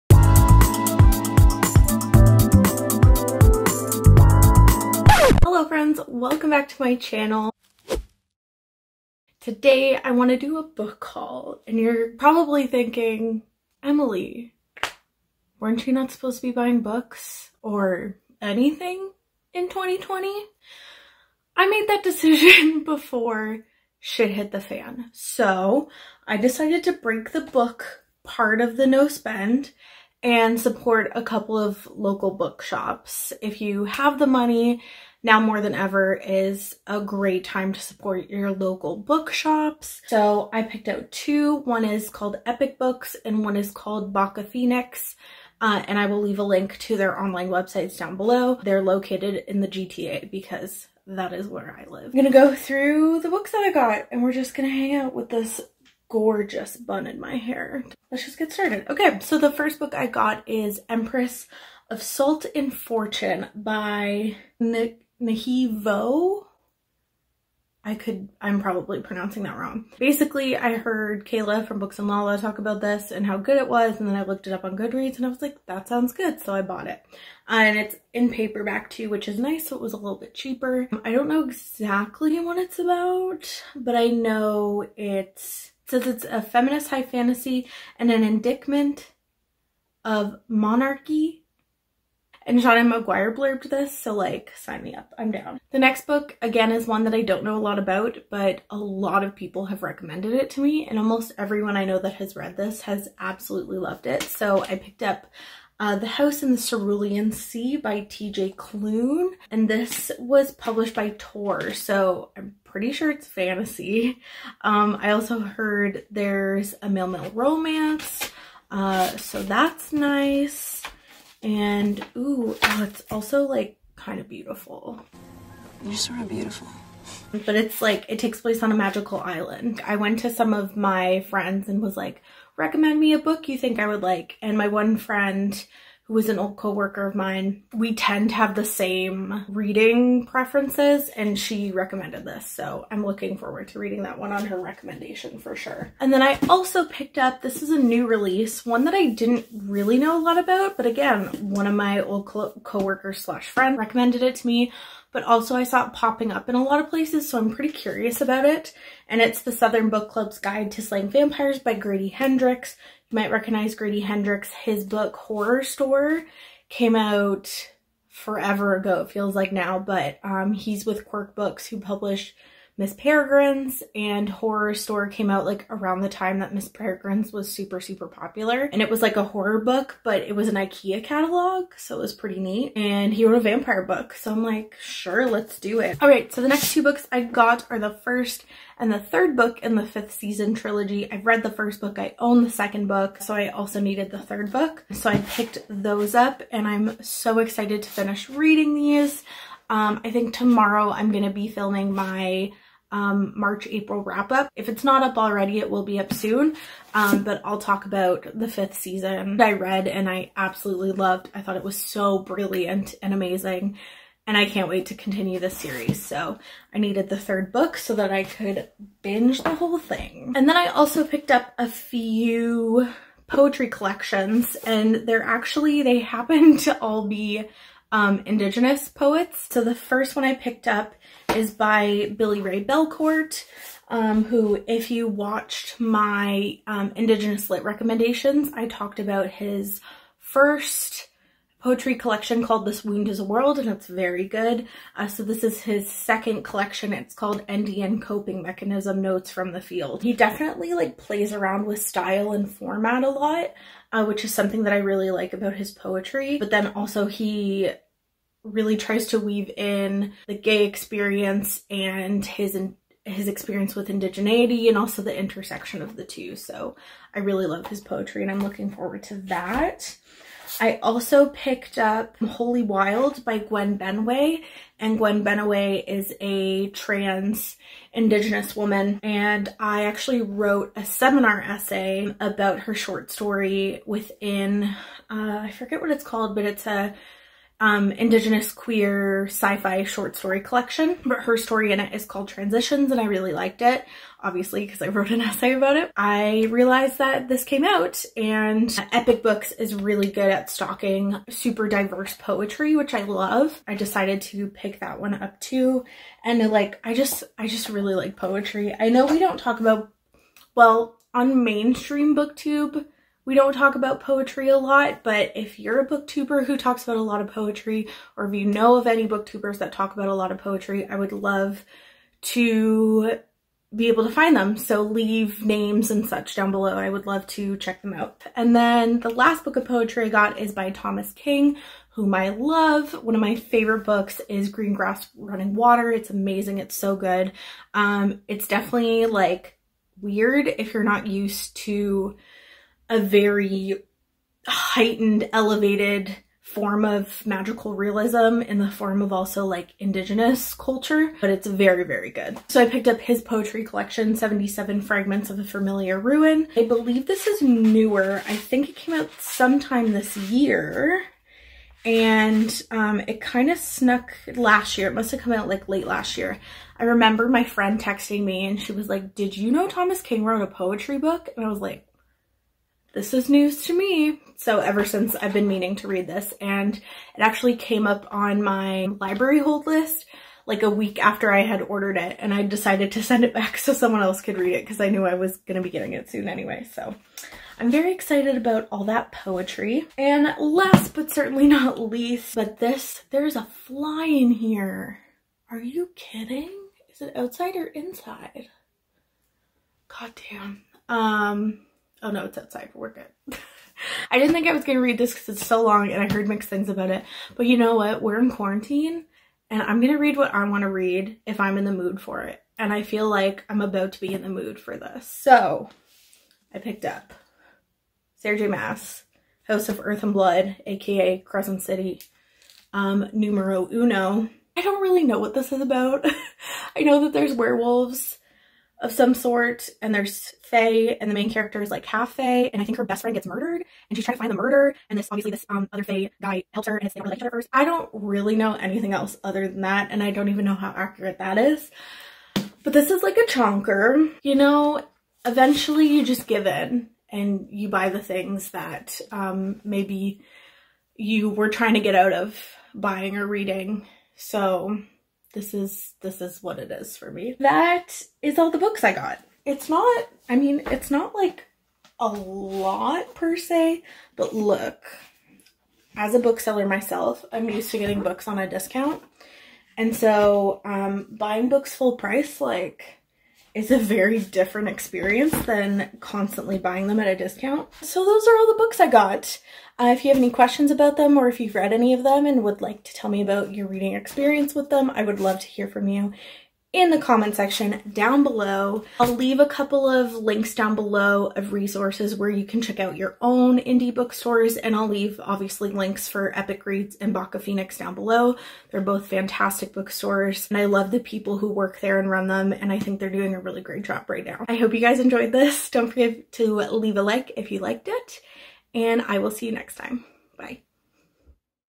book haul: I broke my no spend to support indie bookshops! [cc]
I plan on making lots of Twilight content in the future!
This was filmed a bit ago now, but in these past days, we got some pretty hard news about a member of our family. Thus, I am not quite up to being peppy and filming a video. I'm not about to bring y'all down while we're already grieving our 'normal' lives. I hope you're all doing well and know that if you're also going through a hard time, I feel you, and I'm with you.

If you live in the GTA check out these local bookstores!
~find indie bookstores:
US and Canada: